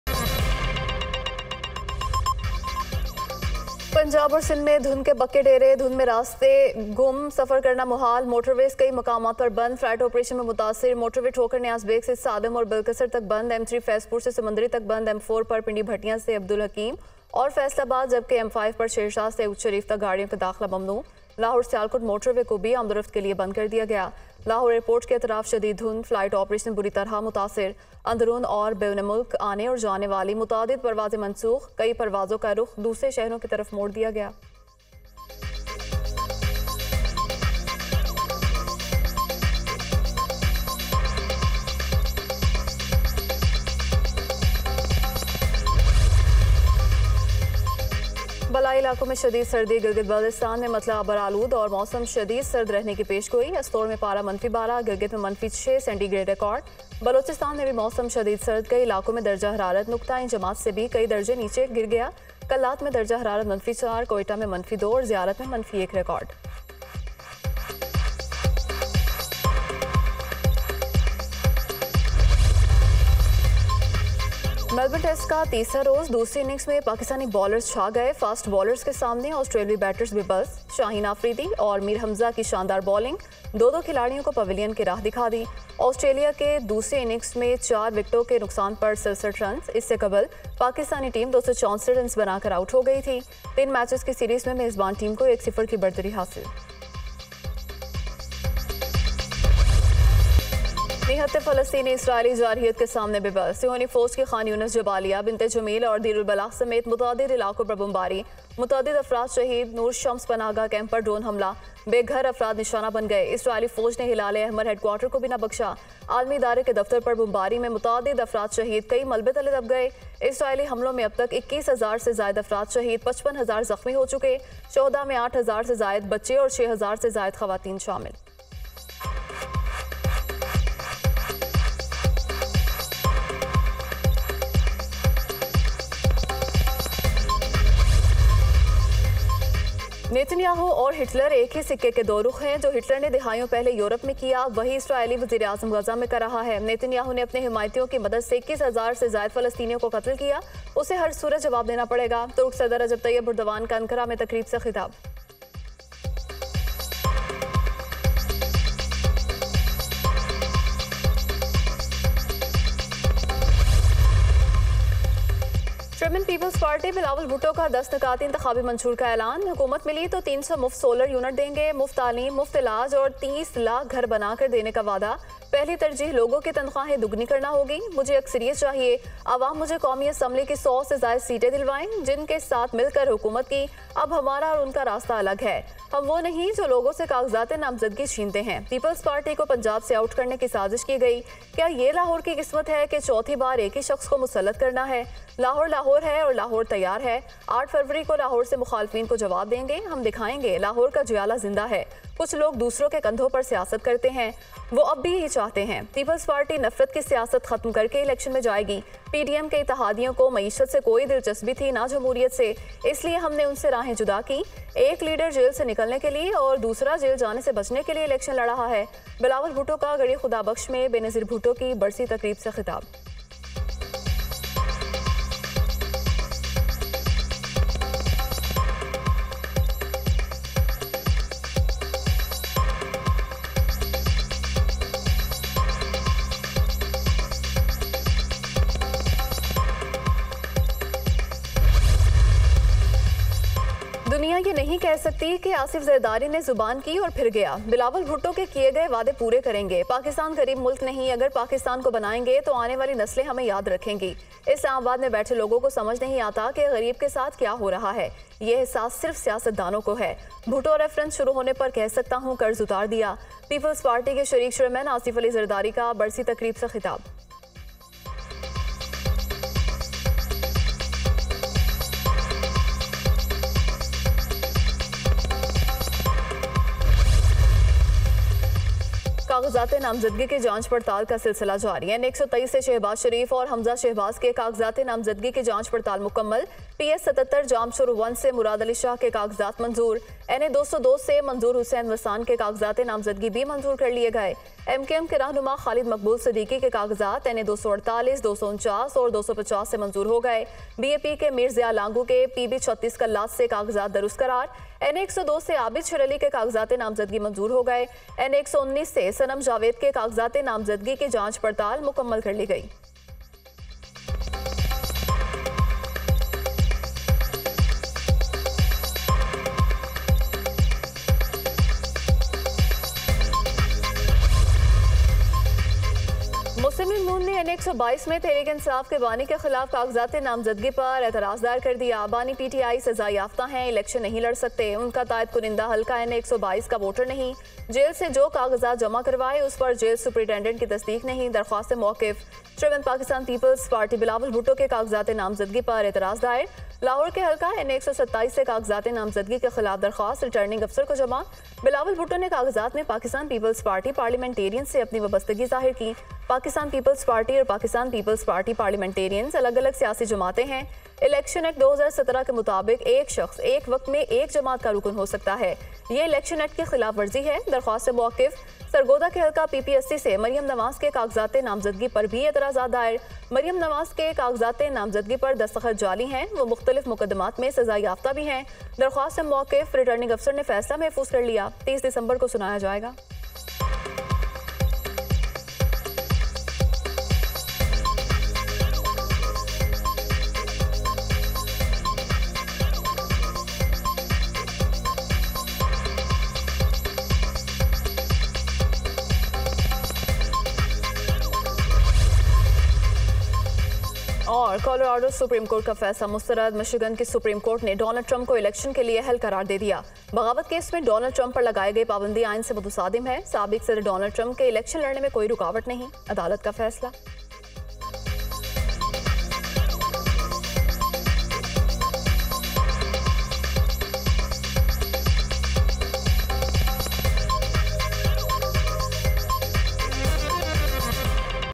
Dunya News Headlines 09:00 AM | Middle East Conflict | 28 Dec 2023
Qureshi arrested from outside Adiala Jail after release on bail

Suspension of disqualification doesn't mean relief in punishment: SC

Bilawal outlines PPP election agenda

Kh Asif serves Rs1bn defamation notice on Usman Dar, his mother

Fog engulfs plains of Punjab

KSE-100 rallies nearly 3pc, Asian stocks up with rate cuts in sight